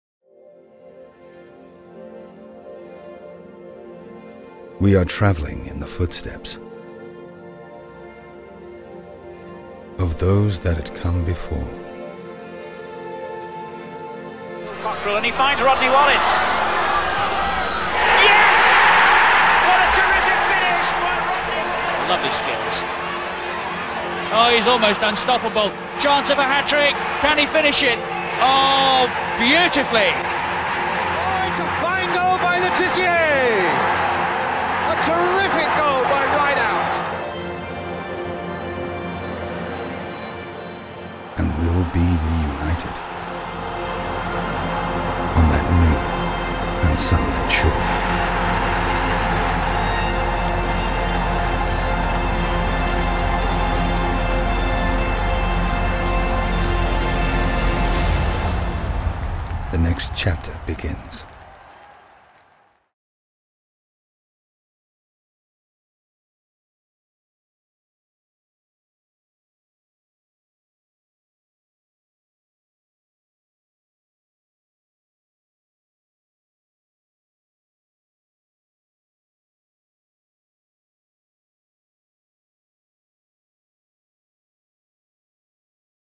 HUMMEL X SOUTHAMPTON
After 30 years apart, we’re extremely proud and honoured to be back as an official kit partner of Southampton FC. It's time for the next chapter!